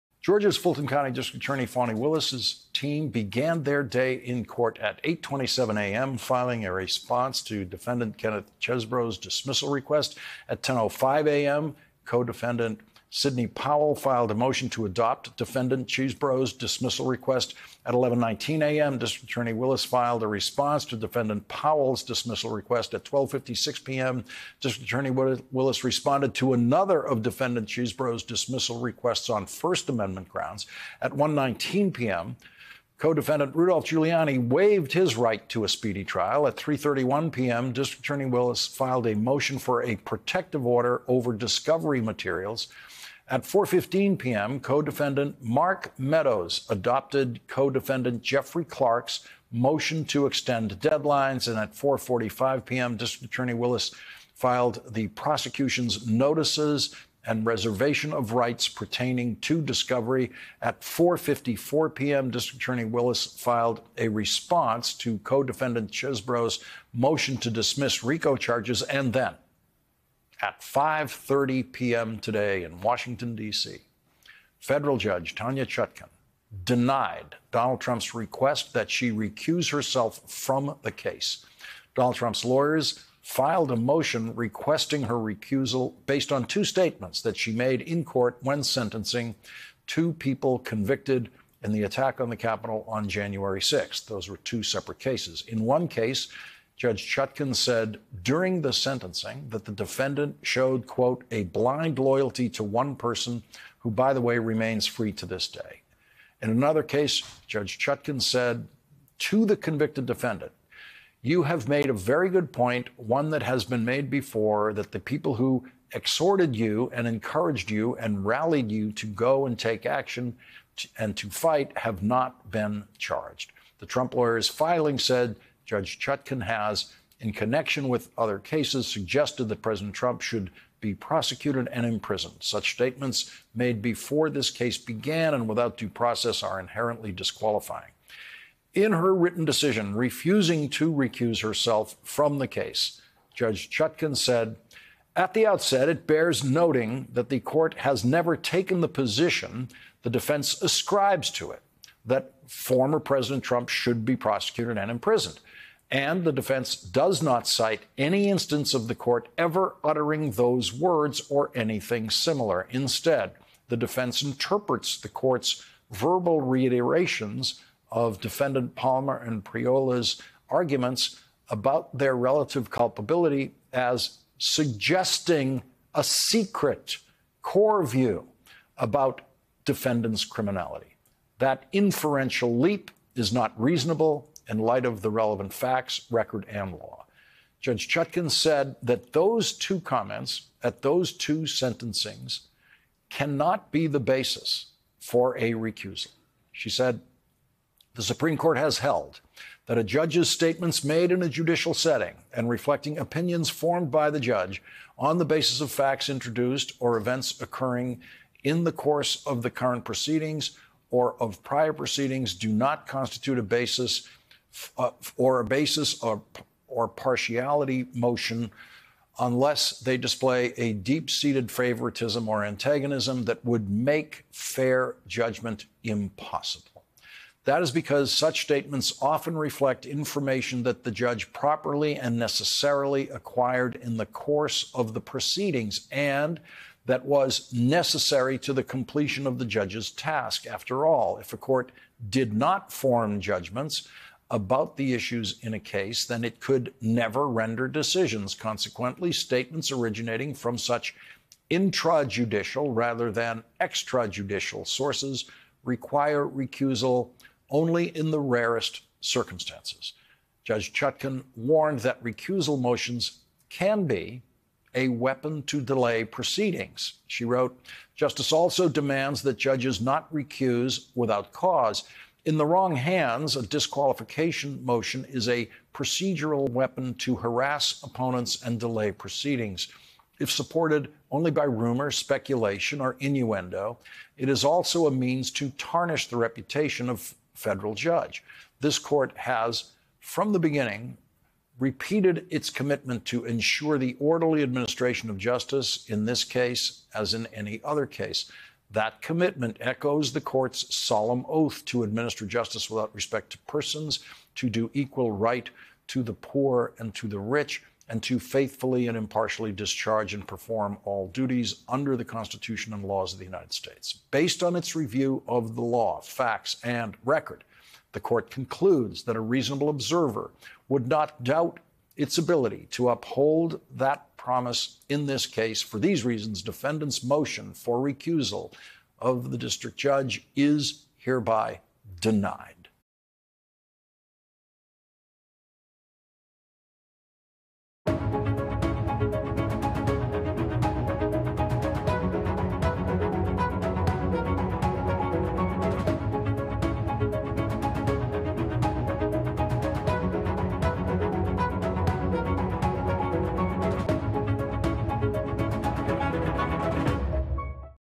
Lawrence: Judge Chutkan denies Trump’s request to remove her from Jack Smith’s 2020 election case
MSNBC’s Lawrence O’Donnell analyzes the decision from Judge Tanya Chutkan to not recuse herself from Special Counsel Jack Smith’s 2020 election case against Donald Trump, as well as the latest developments in the Fulton County District Attorney Fani Willis’ case against the former president and his allies.